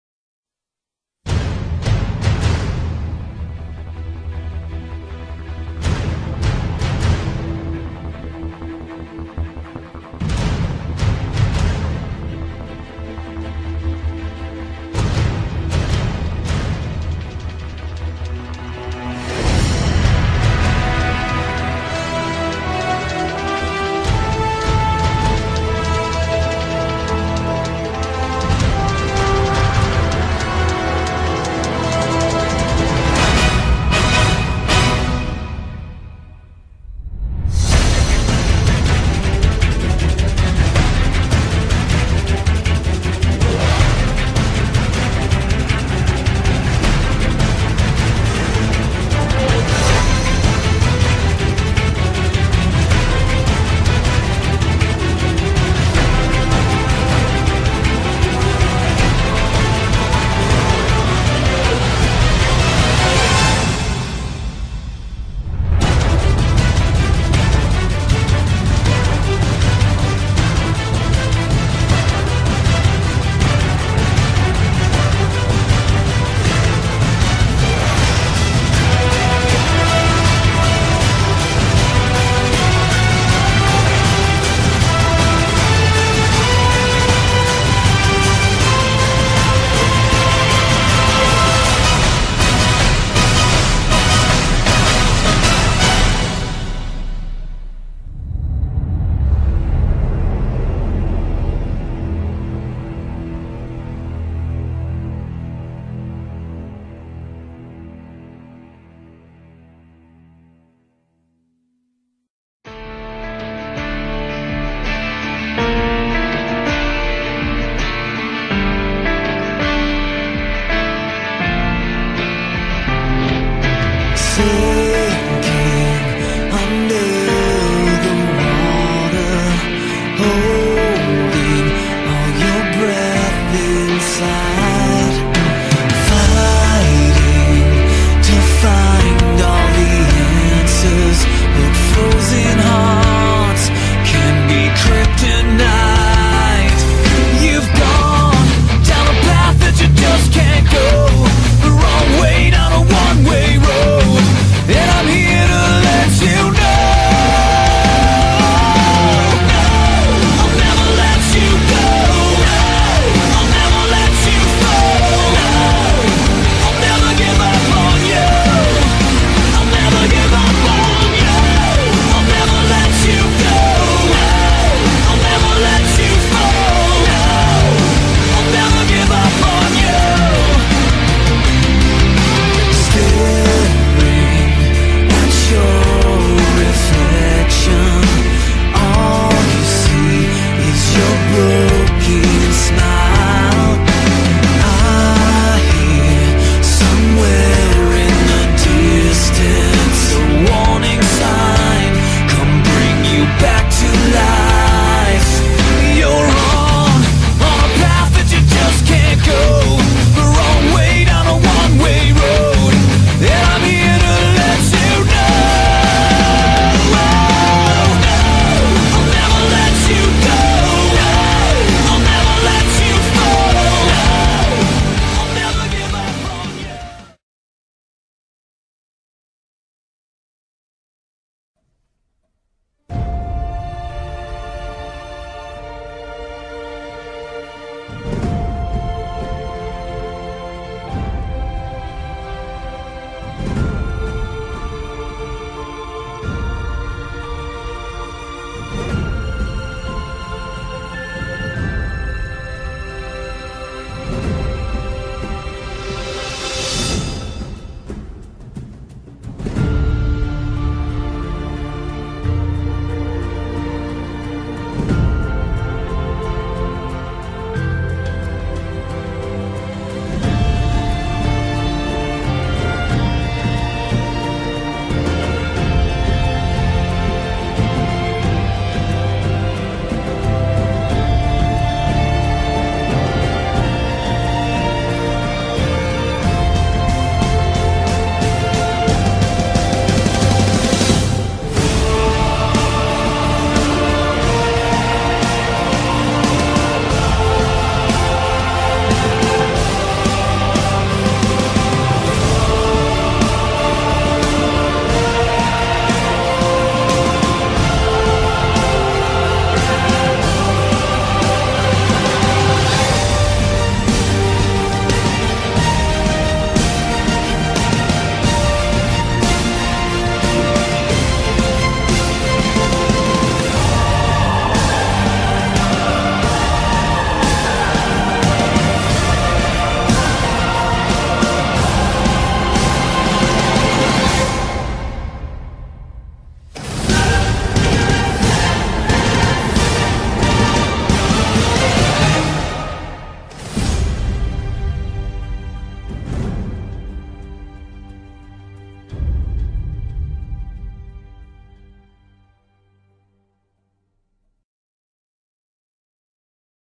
Speedart#5 (DareRising) Manipulation!
Here is a nice manipulation, Not for anyone particularly. Its really just to brush up my skills cause Im trying to get into a decent clan. please comment "Nipples" if you actually enjoy some of the shit I put out: )